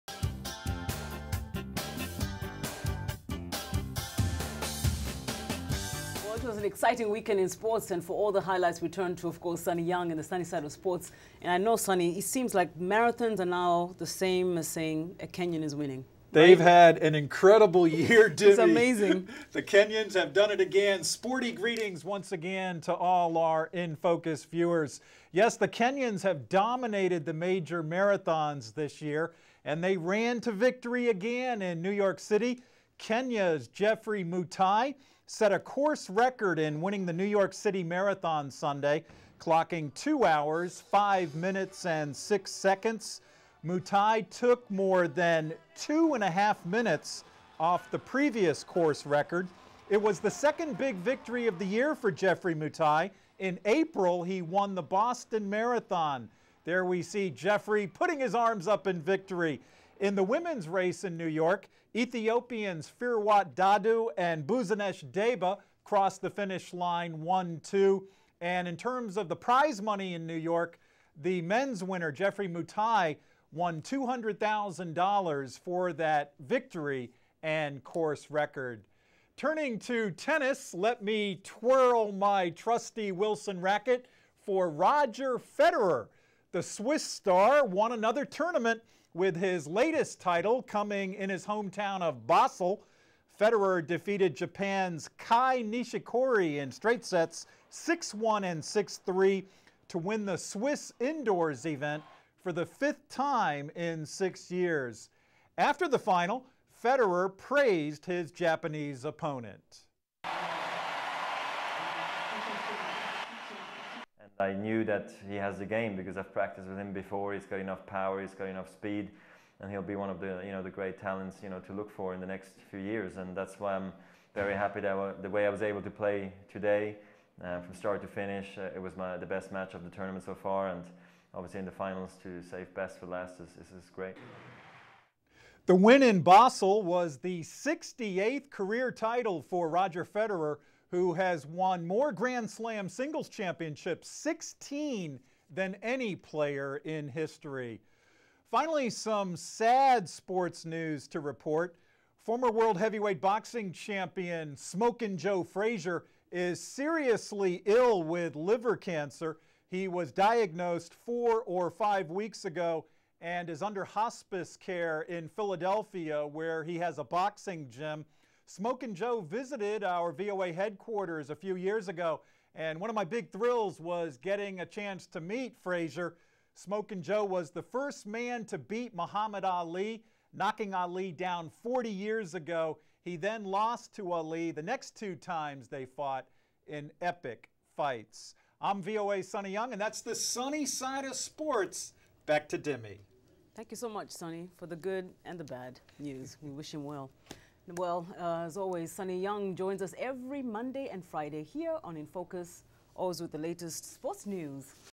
NYC Marathon and Tennis Update
VOA's Sonny Young gives the weekend sport highlights on the NYC Marathon, Federer tennis win and boxer Joe Frazier's health.